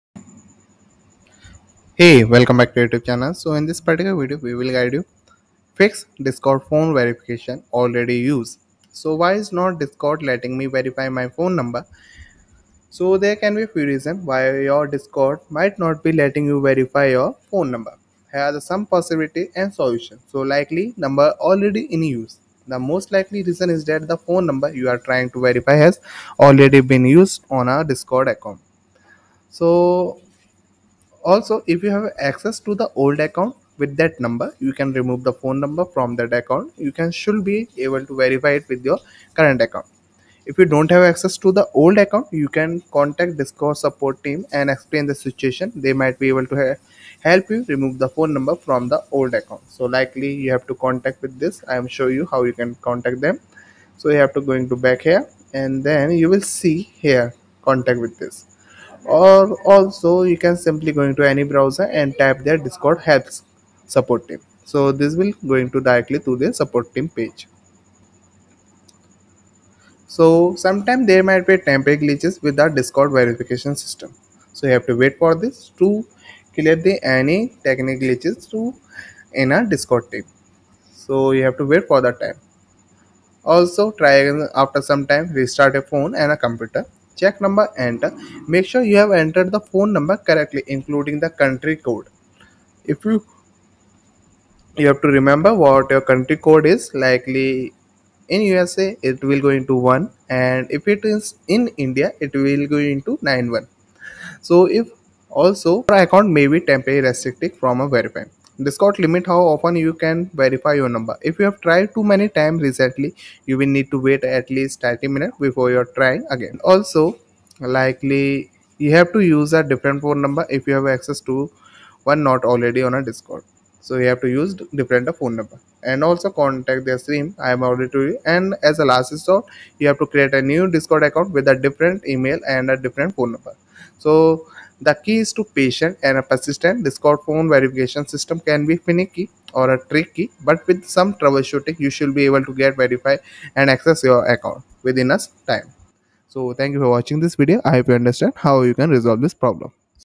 How To Fix Discord Phone Verification Already Used(Why Isn't Discord Letting Me Verify My Phone No.)
In This Youtube Video, We Will Guide You On How To Resolve The Issue Of Discord Phone Verification Already Being Used. If You're Facing Difficulties Verifying Your Phone Number On Discord And Wondering Why It's Not Allowing You To Do So, This Tutorial Is For You. We Will Explain The Possible Reasons Behind This Problem And Provide Step-By-Step Instructions To Fix It. By Following Our Guidance, You'll Be Able To Successfully Verify Your Phone Number On Discord And Enjoy Uninterrupted Communication With Your Friends And Communities.

00:00 Introduction
02:00 Fix 1: Use Different Phone Number